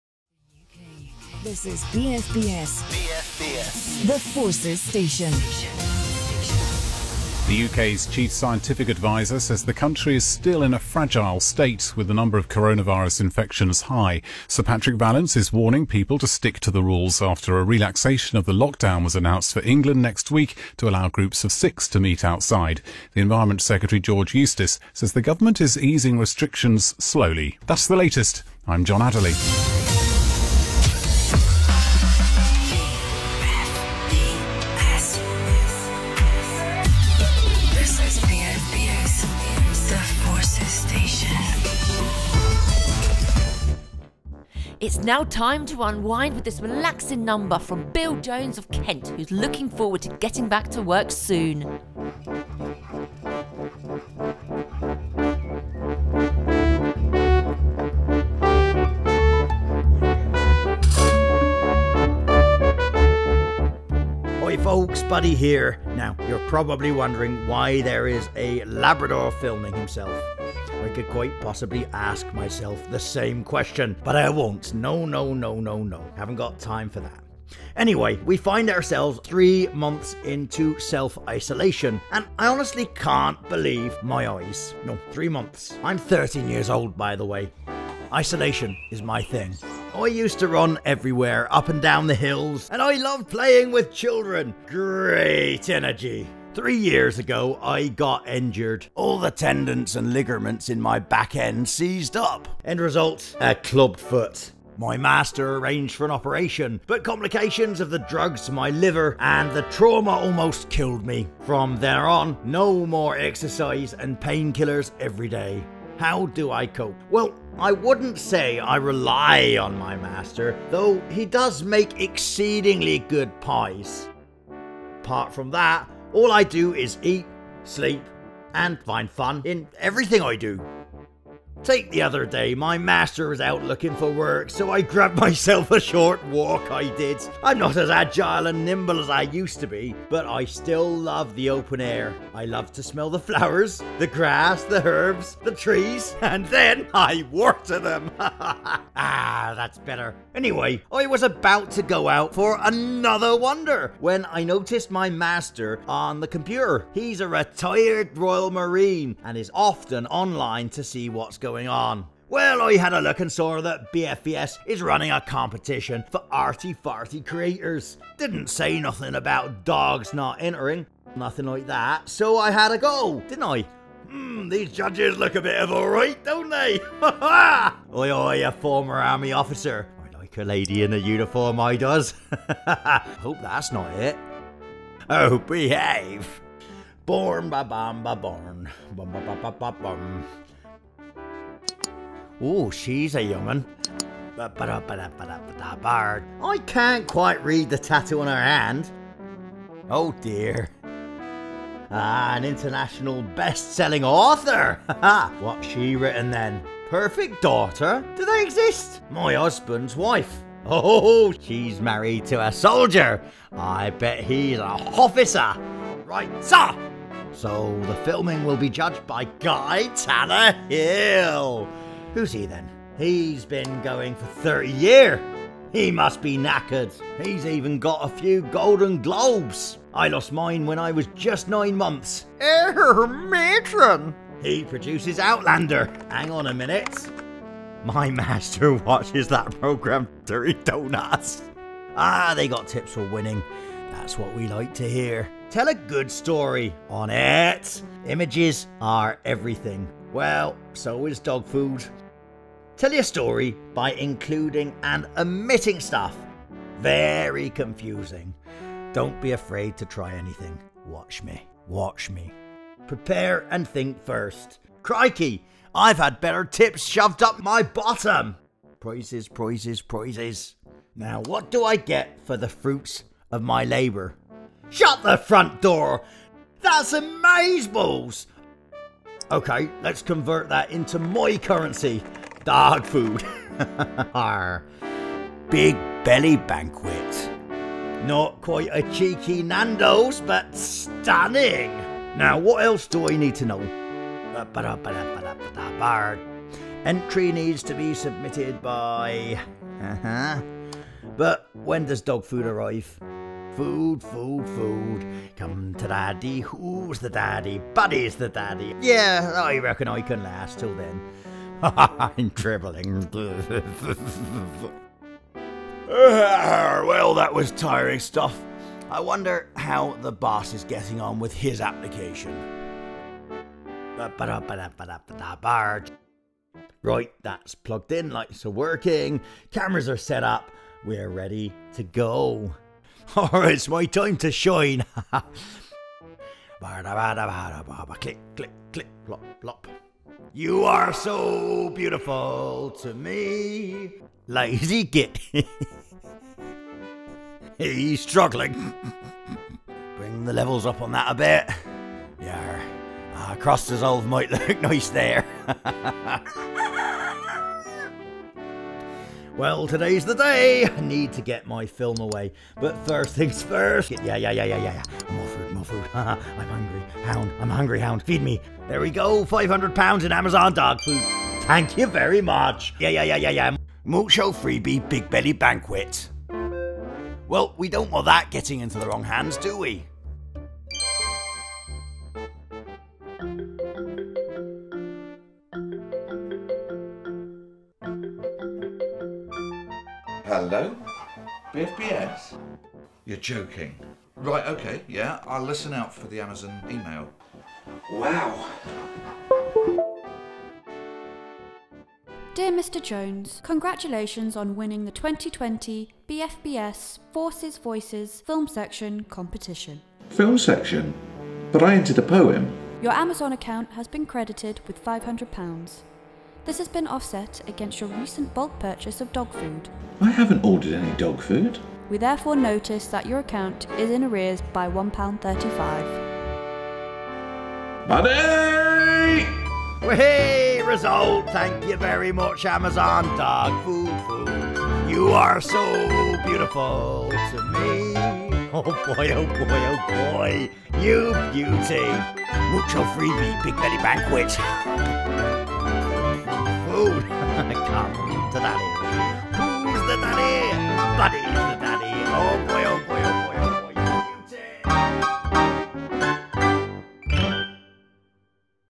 The Competition - BFBS 2020 Film Winner!
In order to win a BFBS film competition, Buddy Jones needs to come up with quite a unique film to inspire the judges.
Full of canine capers, this film short highlights the dexterity of dogs and their ability to shoot Hollywood-quality movies!
Stand back Tarantino!
Judged by the acclaimed TV and Film producer, Guy Tannahill, "The Competition" was awarded first place in the 'actual' competition!
Written, filmed, edited and produced by Matt Jones.